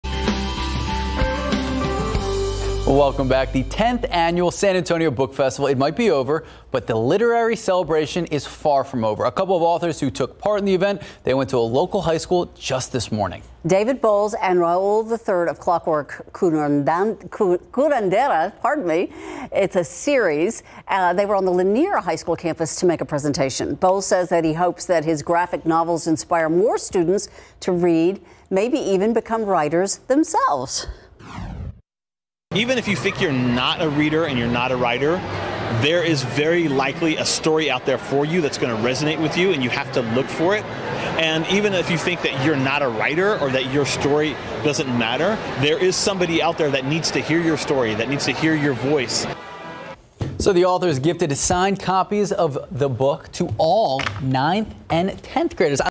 San Antonio Book Festival authors visit Lanier High School
David Bowles and Raul the Third of "Clockwork Curandera" series gifted signed copies of the book to all ninth- and tenth-graders at Lanier High School.